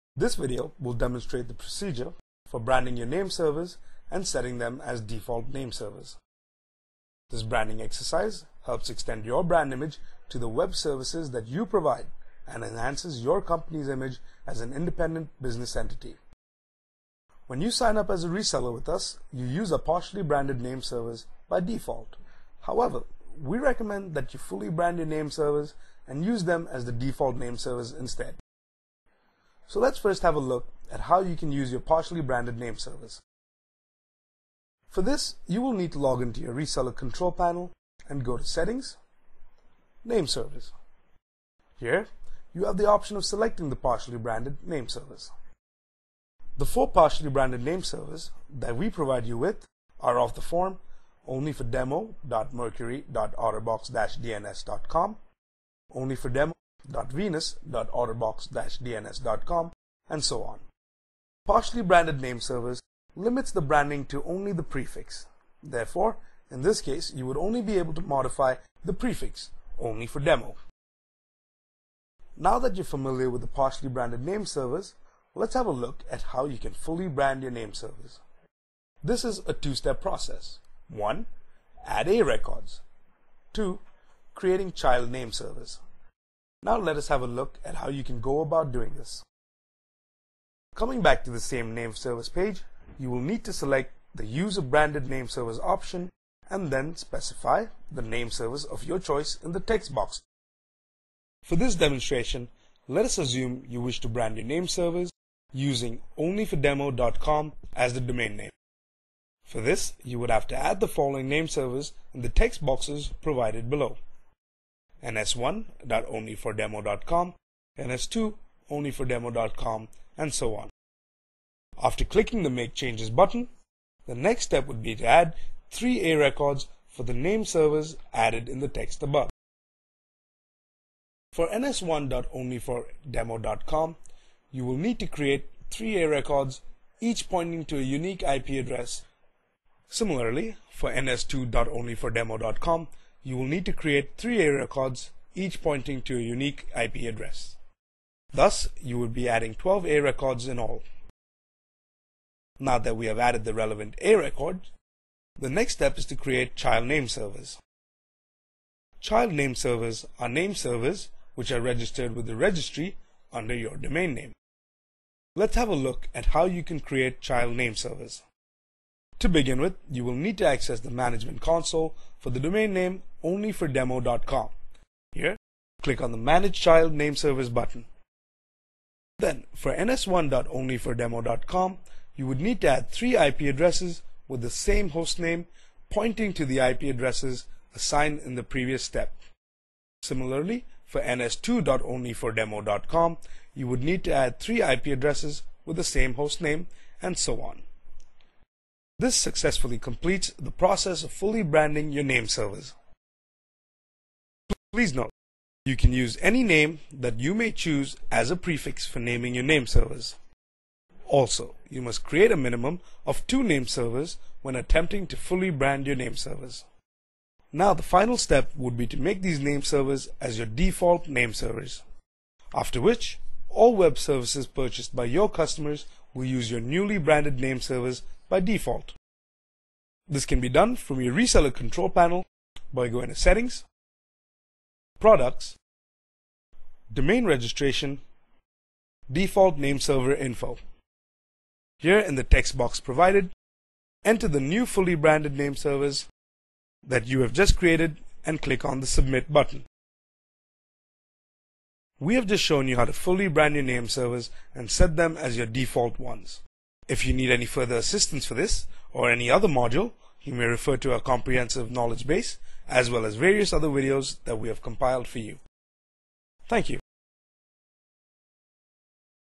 Instructional Video: Branding Nameservers
Learn how to brand Nameservers with your company name/domain using the ResellerClub Control Panel.